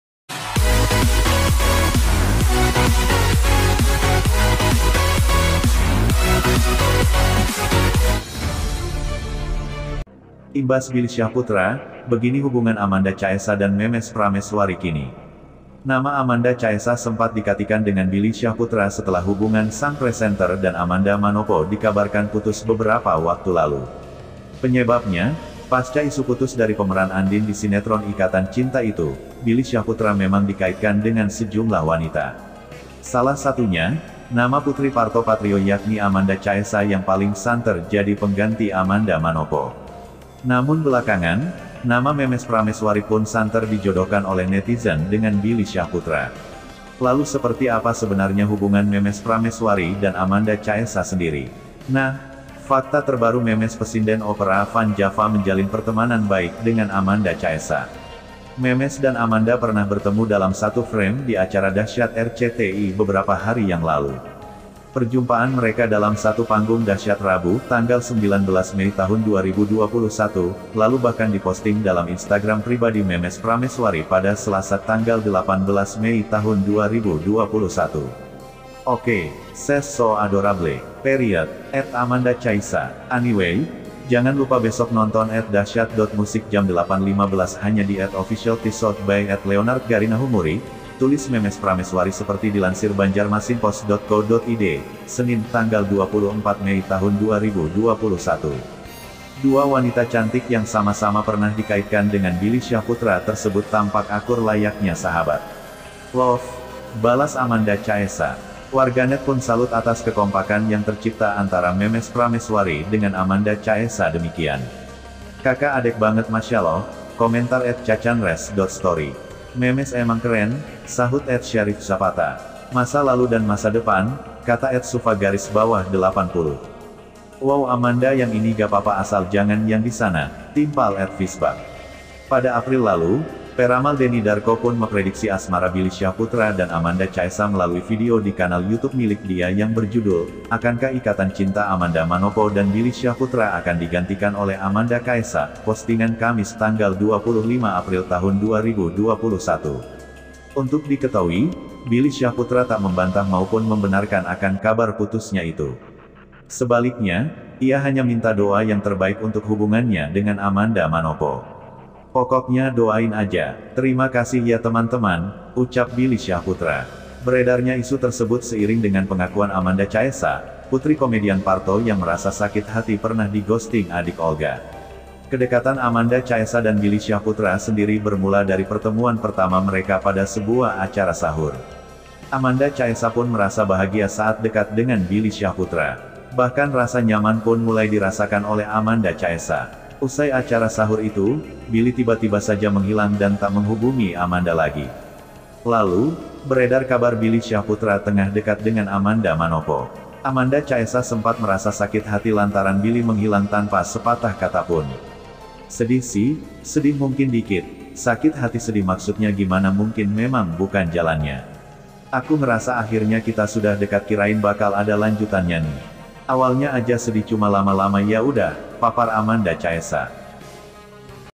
Imbas Billy Syahputra, Begini Hubungan Amanda Caesa dan Memes Prameswari Kini
Nama Amanda Caesa sempat dikatikan dengan Billy Syahputra setelah hubungan sang presenter dan Amanda Manopo dikabarkan putus beberapa waktu lalu.

Penyebabnya, pasca isu putus dari pemeran Andin di Sinetron Ikatan Cinta itu, Billy Syahputra memang dikaitkan dengan sejumlah wanita.